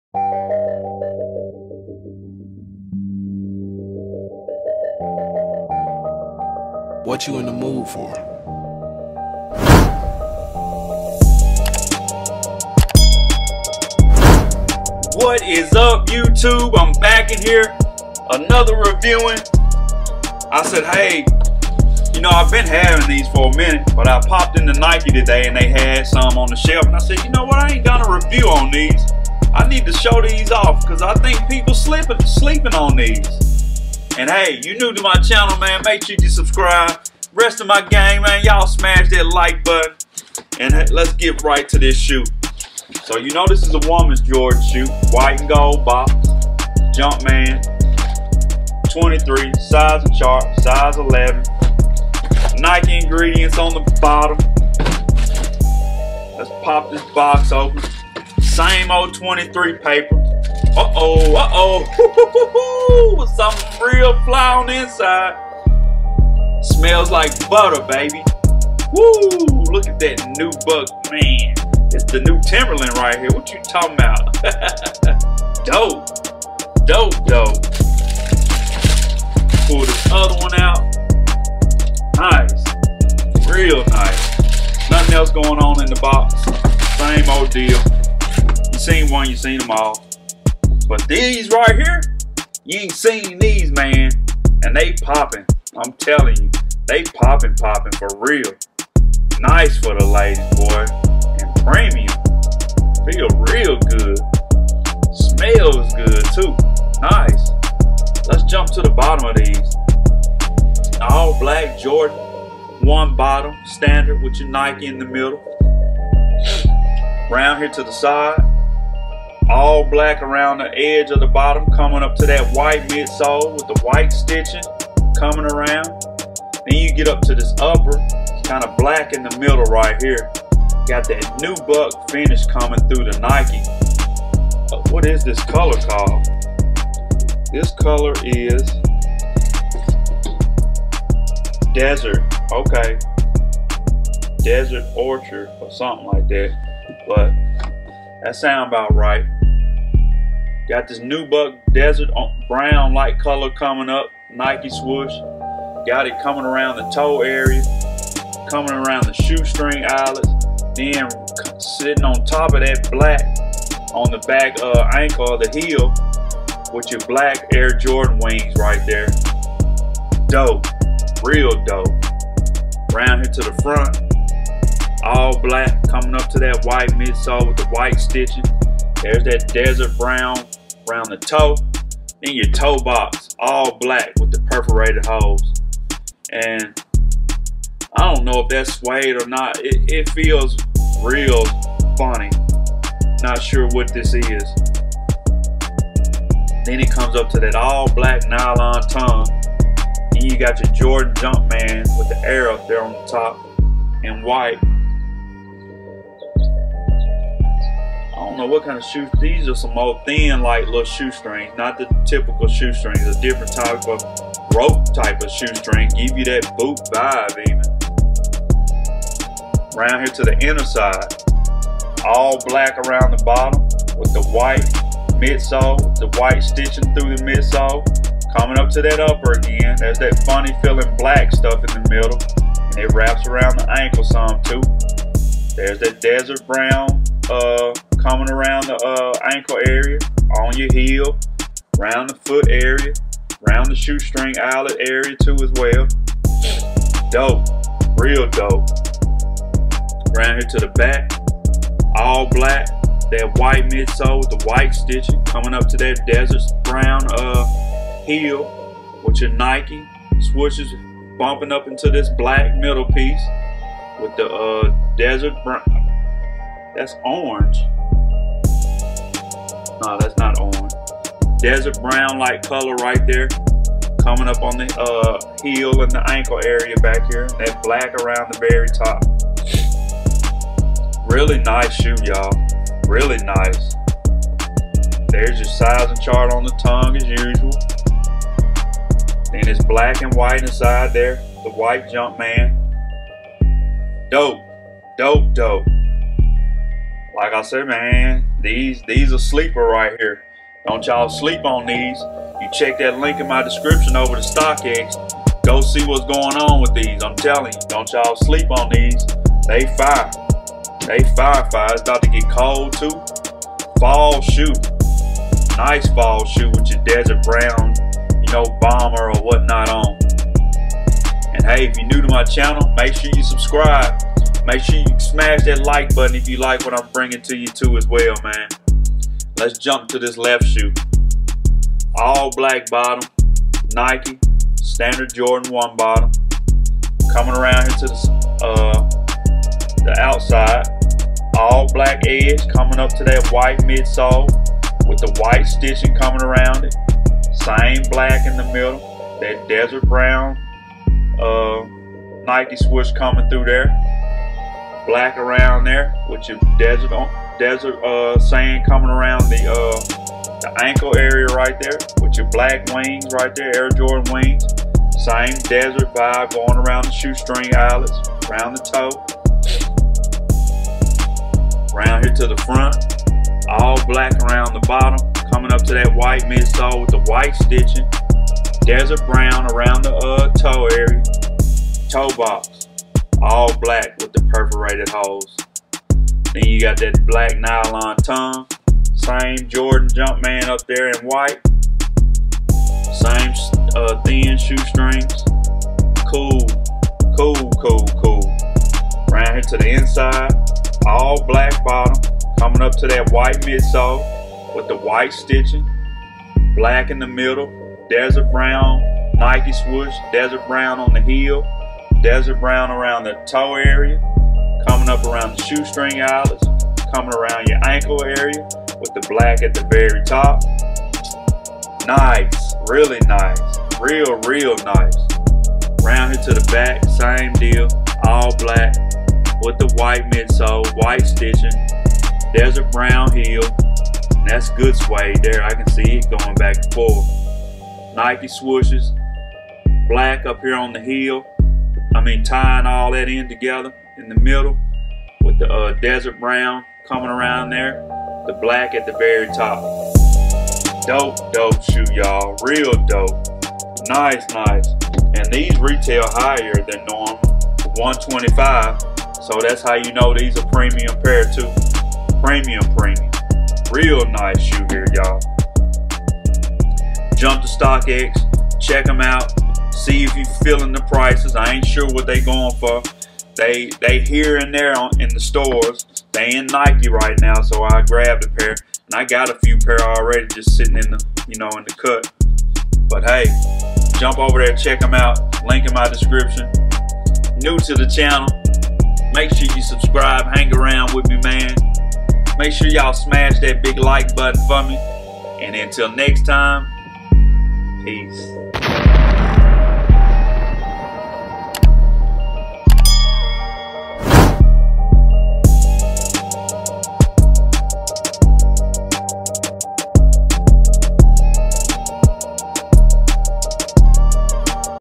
DON'T SLEEP ON THESE AIR JORDAN 1 WHEAT ( Desert Ochre BLACK) 2020 !!!
This video is about AIR JORDAN1 MID WHEAT COLOR ( Desert Ochre) FULL REVIEW AND WHERE TO BUY.